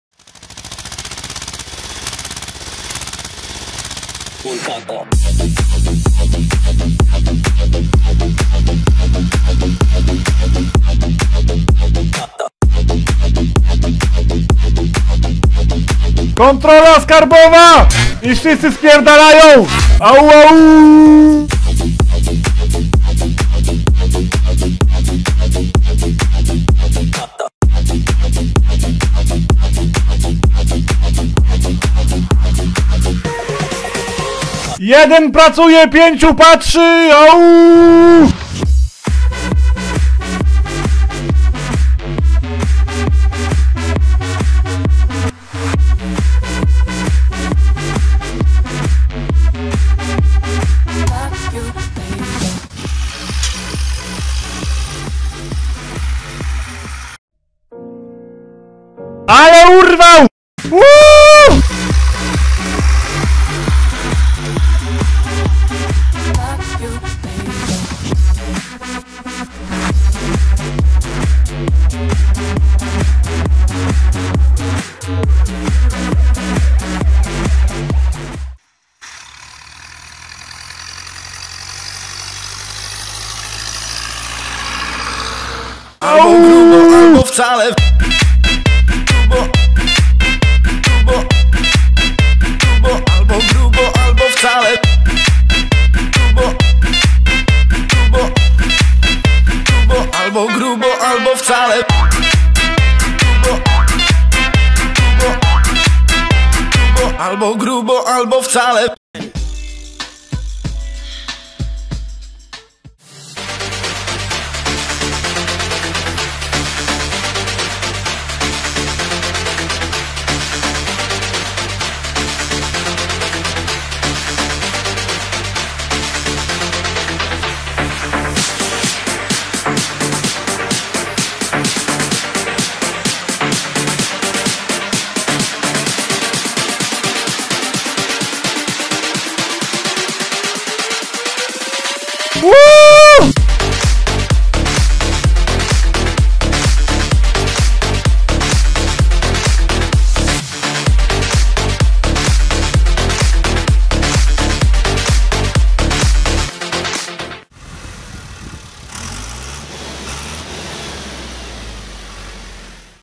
Sianokosy z Ekipą UuAA [2xZetor,Renault,Ursus,Claas,Fahr] w akcji Aauuuu
Witam ;)
W filmie występują:
Renault Ceres 330X & Claas Rollant 46
Zetor 4340 & Przyczepy ;)
Zetor 5341 & Kosiarka Rotacyjna
Ursus c330M & przetrząsarka i zgrabiarka Fahr
Jeśli spodobał Ci się film zostaw coś po sobie, ponieważ to naprawdę mnie motywuje do dalszego działania.
Film nagrywany w 2017 r. Drugi pokos ;)